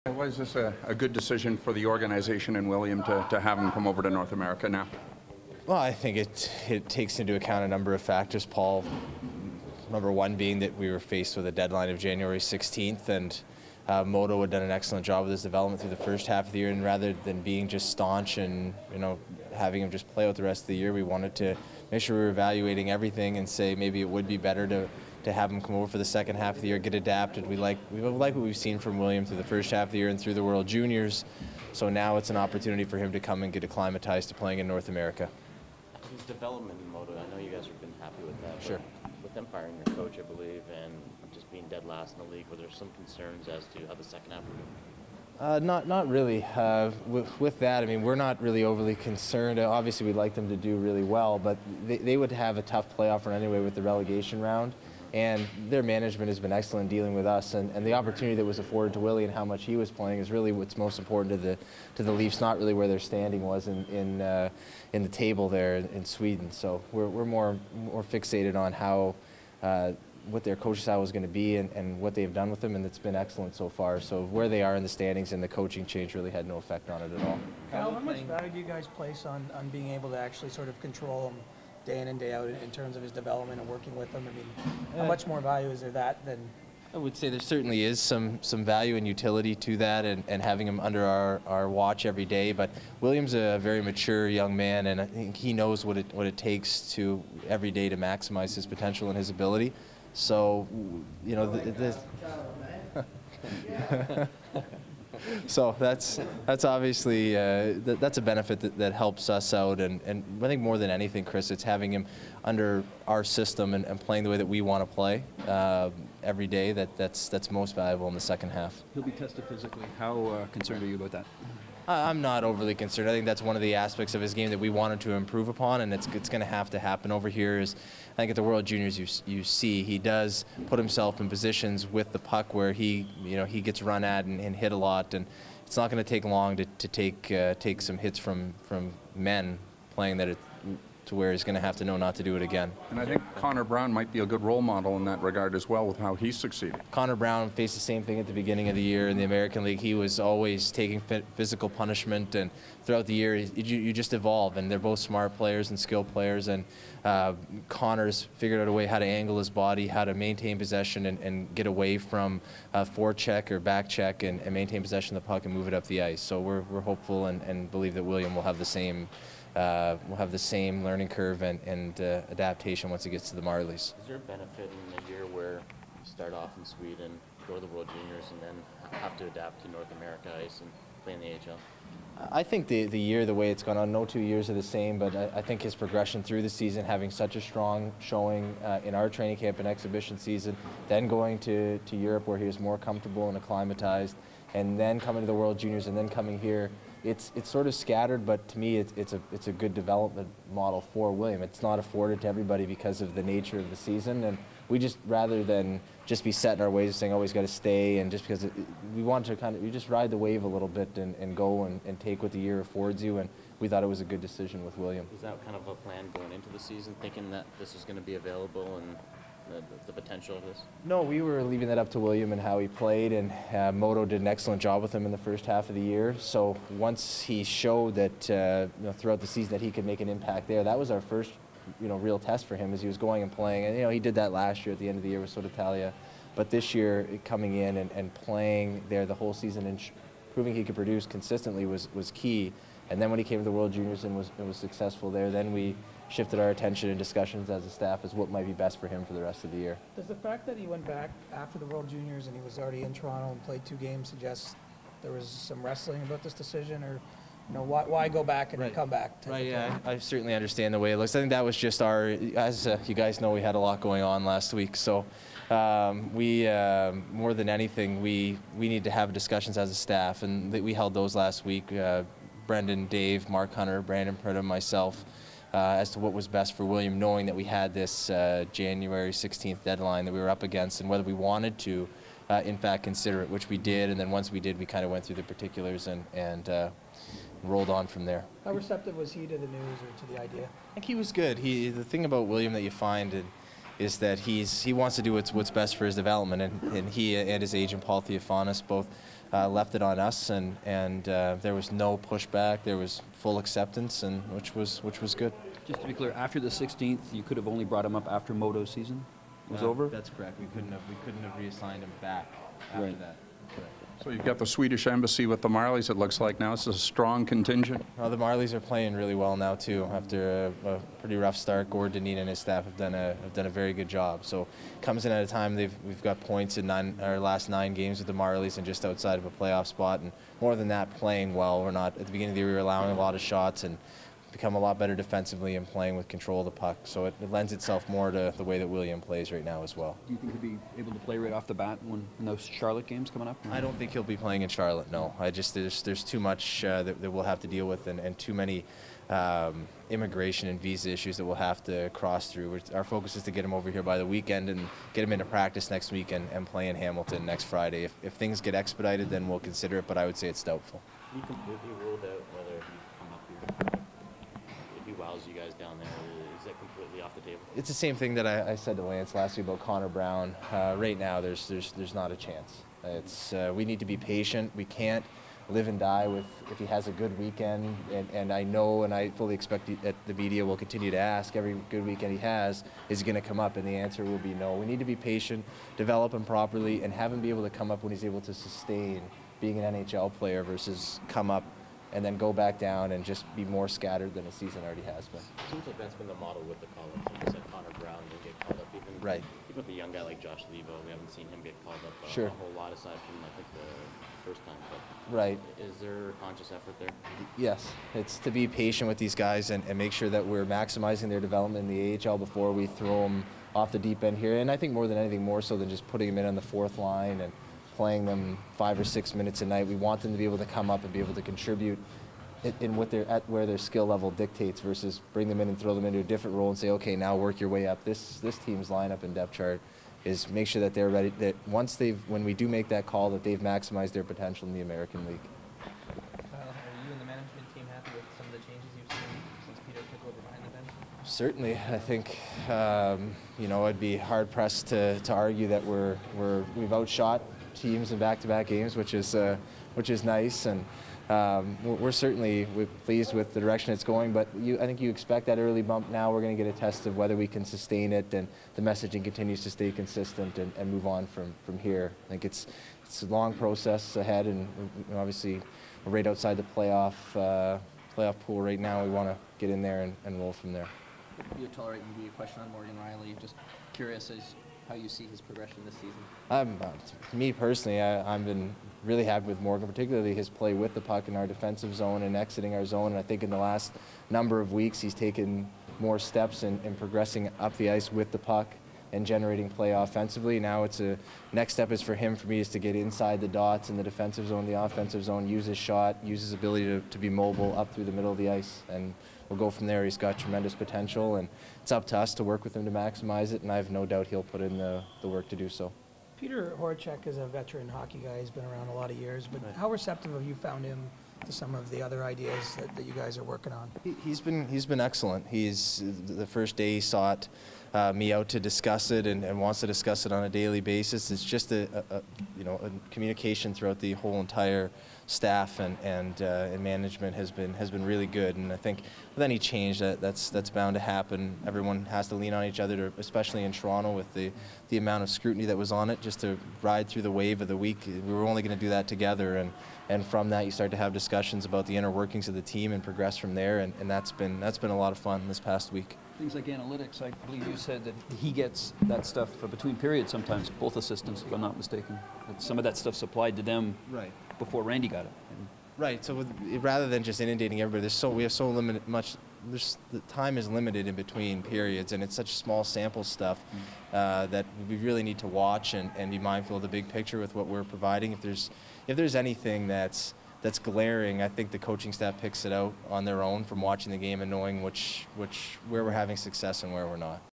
Kyle Dubas - January 12, 2015
Kyle Dubas speaks with members of the media regarding William Nylander joining the Toronto Marlies.